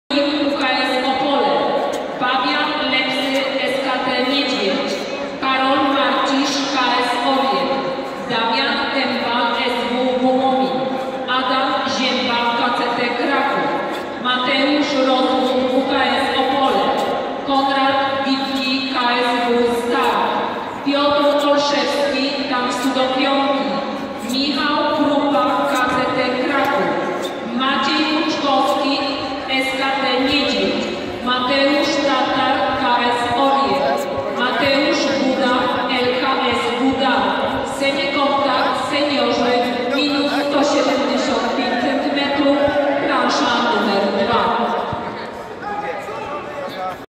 Paulina Stańczak Centuria Toruń układ (Mistrzostwa Polski PUT Taekwon-Do ITF Częstochowa)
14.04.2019